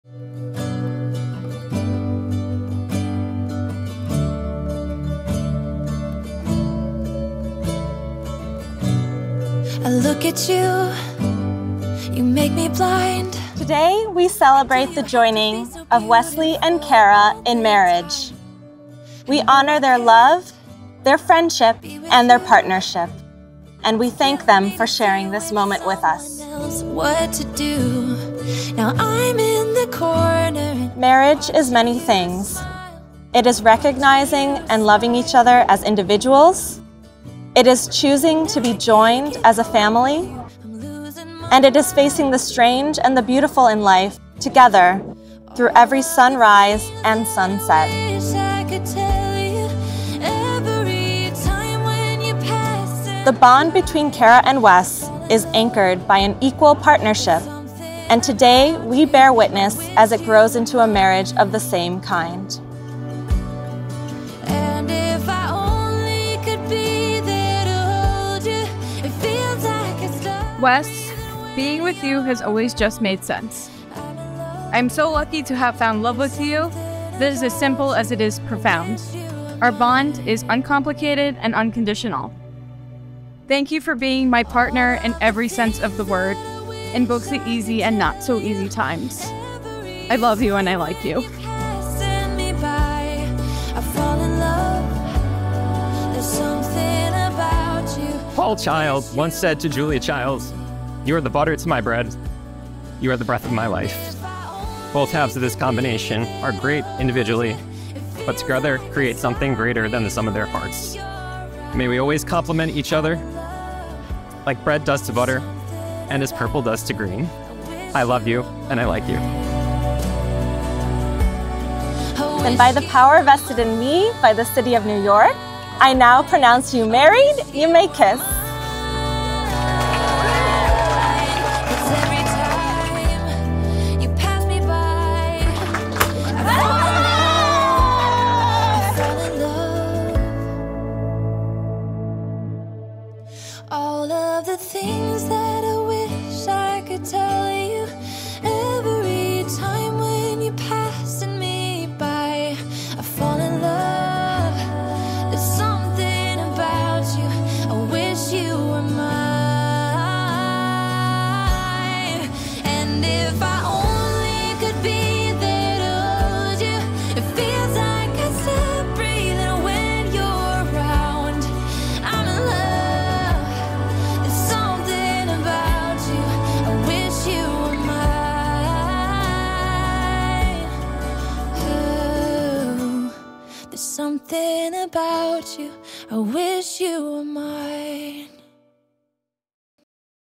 Wedding At Cop Cot With the Couple's Friend Officiating
Kara & Wesley chose our All Inclusive Package with Extra Hour of Photography before the ceremony, 2 Non Bridal MUAH services, extra Musician and Zoom Live-streaming. They decided to do their ceremony at one of the biggest location of Central Park -Cop Cot.